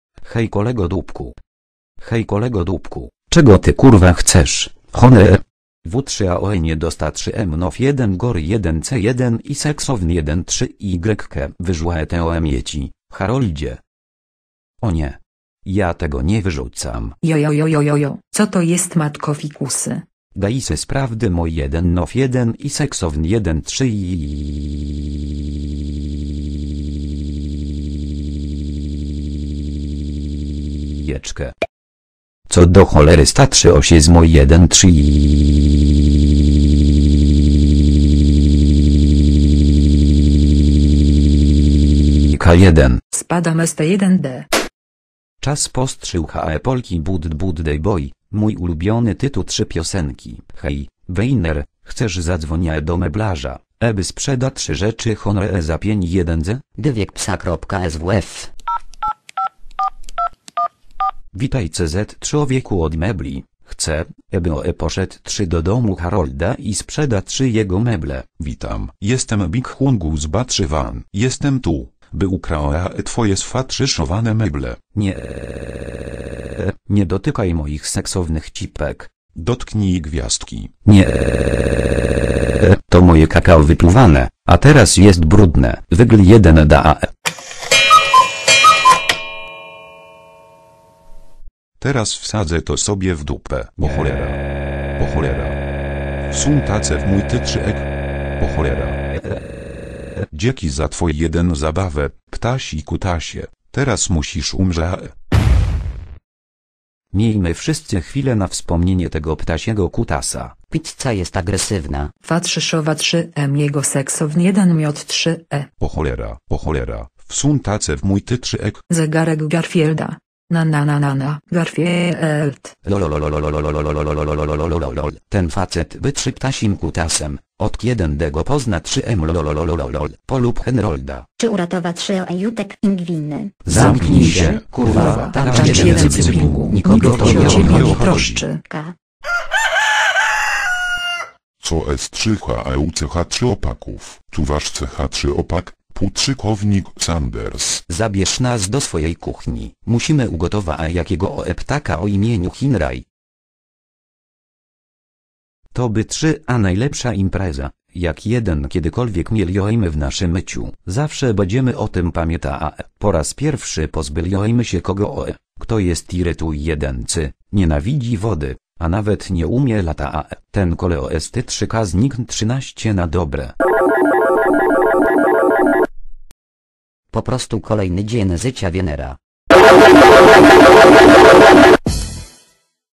Honree Gets a Noo Spewn and Dies (Fanmade Polish Dub)
Someone from Poland who was a fan of Lenstar's Oswald videos decided to make a fanmade Polish TTS dub of it.

However their channel got terminated after Poe Lar Bear got offended that Polish People aren't Polar Bears.

Big chungoose the snowman!